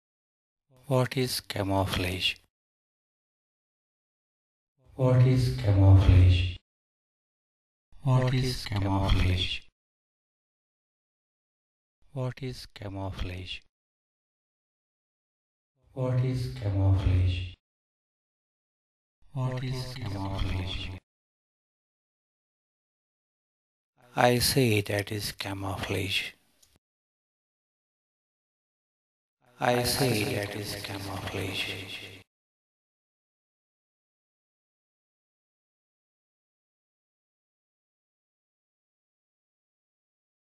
Camouflage -- Leaf Frog
Leaf Frog, Megophrys nasuta, camouflaged in leaves.